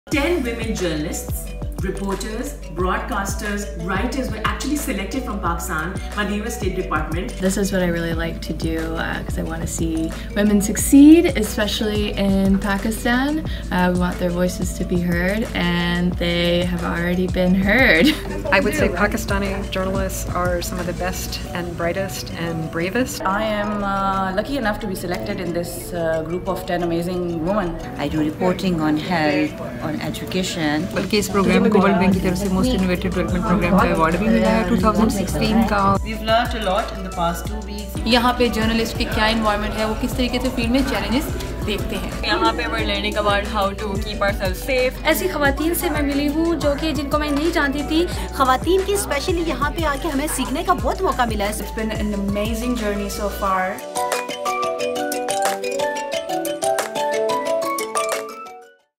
TRIP TO USA | Media Training Program | Promo 2 | The "S" Stories
Watch the 58th episode of the "S" Stories, as we're taking you on a journey to Washington DC & Chicago. I was fortunate enough to be a part of an fantastic training program, along with 10 talented women of Pakistan working in the field of journalism. This program was organised by the US Consulate General Lahore in collaboration with Meridian International Center. We had the opportunity to learn the international journalism practices & also explore the culture, food & people from DC & Chicago. A big shoutout to WorldChicago from my side too.